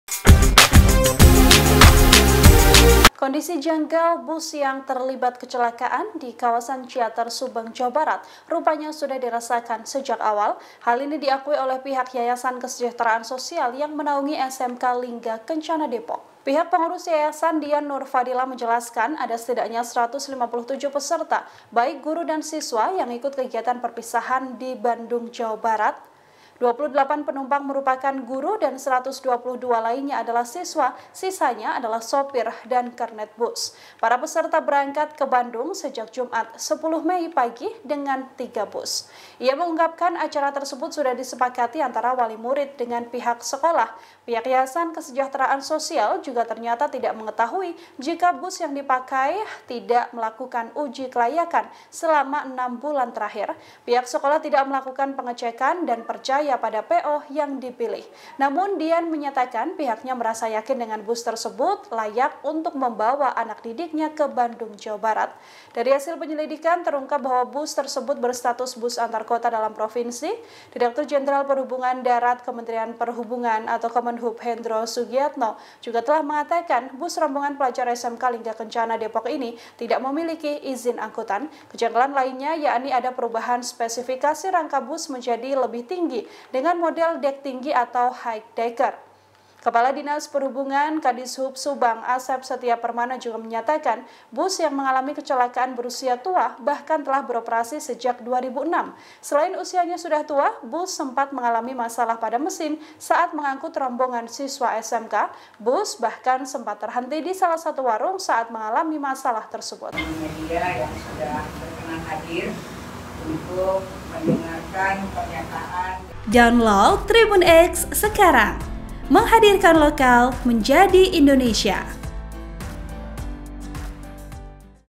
PO Bus Putera Fajar Tutupi Kondisi Janggal Bus dari Pihak Sekolah
TRIBUN-VIDEO.COM - Kondisi janggal bus yang terlibat kecelakaan di kawasan Cianter Subang Jawa Barat rupanya sudah dirasakan sejak awal.

Hal ini diakui oleh pihak Yayasan Kesejahteraan Sosial yang menaungi SMK Lingga Kencana Depok.